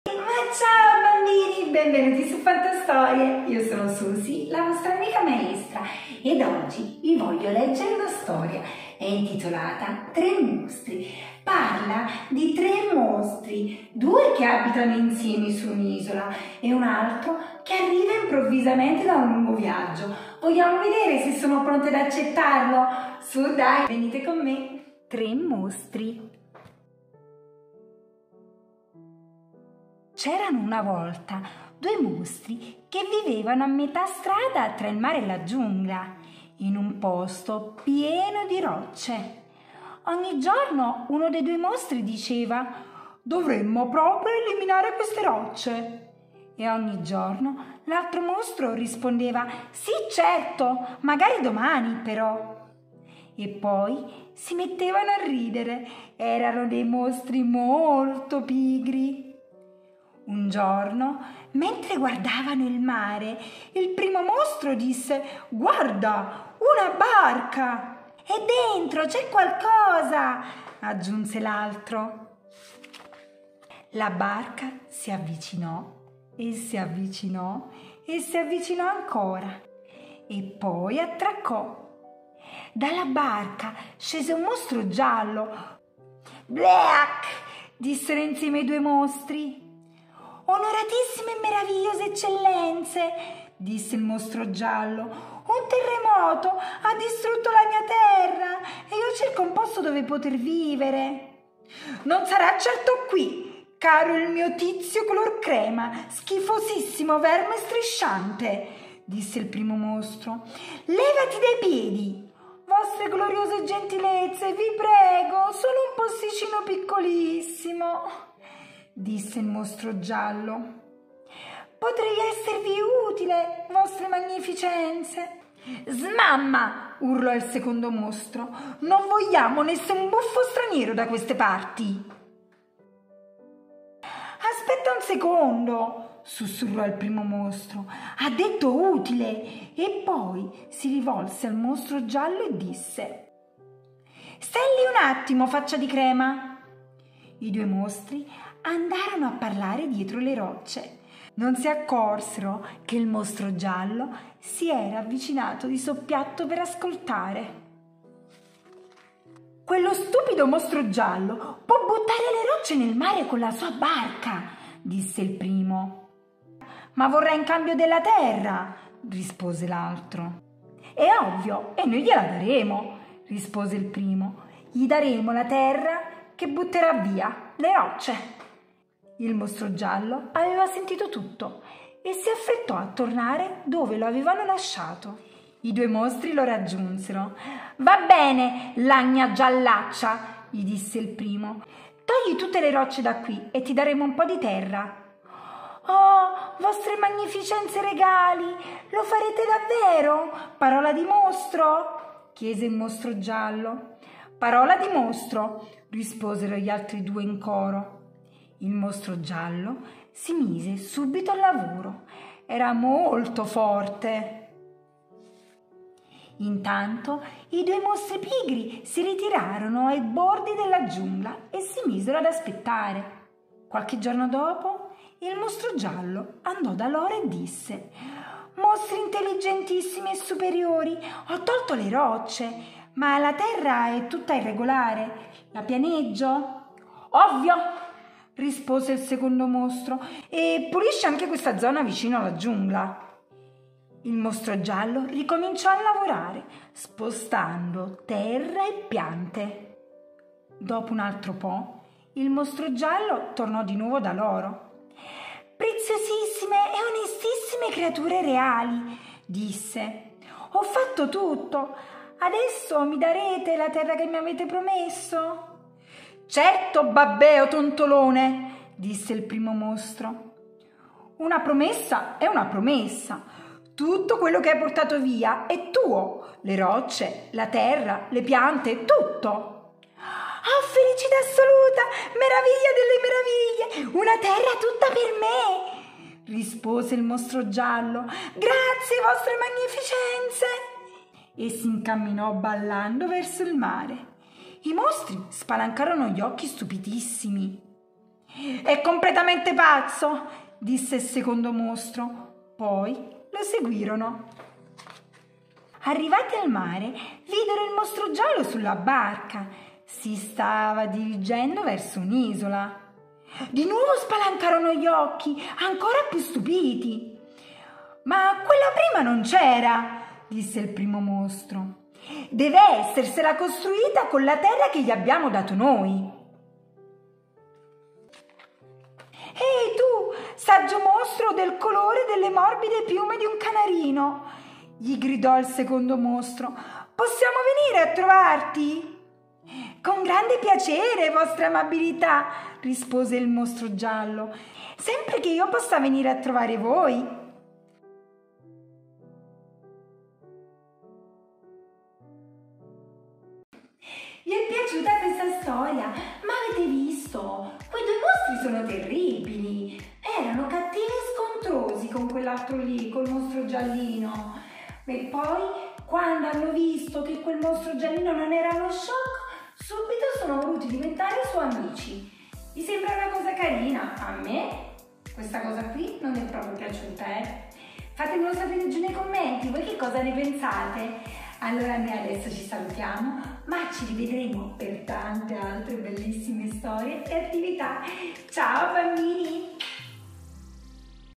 "Tre mostri" di David McKee
"Tre mostri ", albo fratello di " Due mostri" nati dalla penna di  David McKee ,
ci parla di

"rispetto, di popoli, di diversità, di accettazione del diverso".

Ancora una volta ci sono un Mostro Blu e un Mostro Rosso che vivono su un’isola .Un giorno arriva … un Mostro Giallo.Arriva in pace. Arriva con parole gentili, chiede asilo perché un terremoto ha distrutto la sua terra.
lo accetteranno?
 Questa storia parla ai bambini di cose che conoscono molto bene: liti tra amici  e un nuovo arrivato nella compagnia che magari inizialmente non è ben accetto.

Questo albo mosta loro, come uno specchio , le situazioni spiacevoli, ma del tutto normali.
Vuole comunicare tra le righe "  Se vi comportate così, sopraffatti dall’istinto non mediato dalla ragione, vi trasformate in mostri" .

Età dai 4 anni